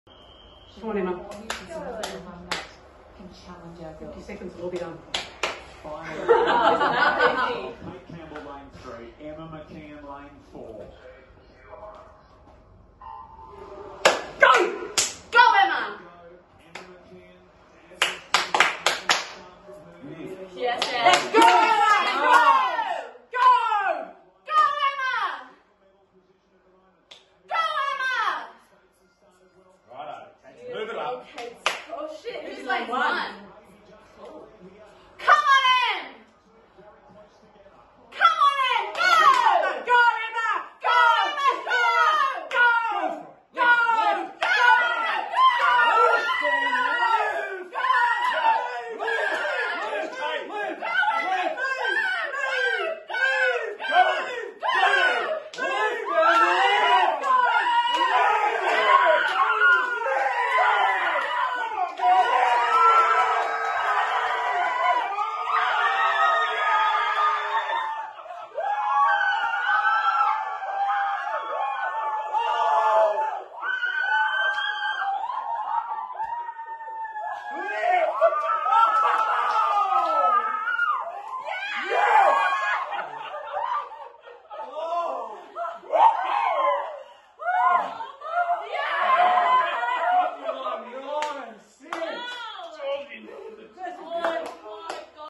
Follow the McKeons on an emotional journey of a lifetime!
This video of Emma McKeon's family watching her 100m Freestyle final at Tokyo 2020 is everything!
🎥 David McKeon